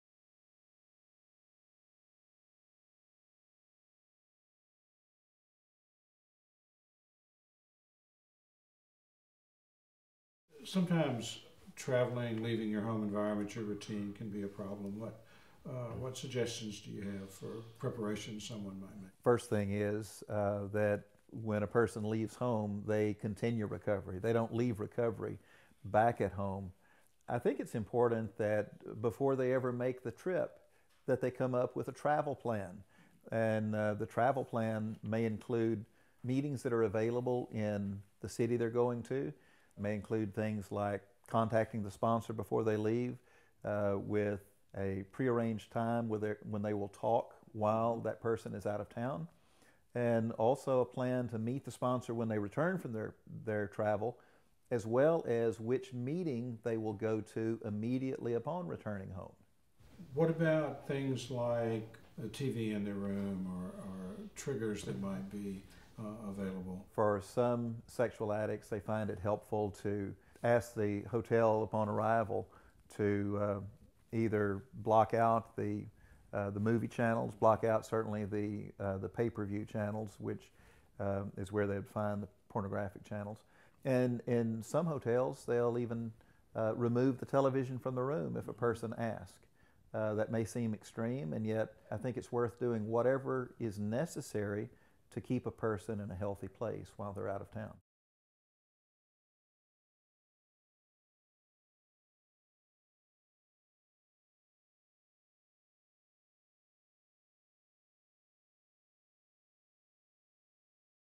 Traveling When In Recovery From Sex Addiction
In this video Dr. Milton Magness asks the questions: How do you continue in recovery from sex addiction when you are out of town? Do you put recovery on hold? Recovery can and must continue. Dr. Magness gives practical suggestions for maintaining strong recovery in spite of travel.

Please send this link to my video, Traveling When In Recovery From Sex Addiction, to friends who might find it helpful.  Or take a moment and post in on social media.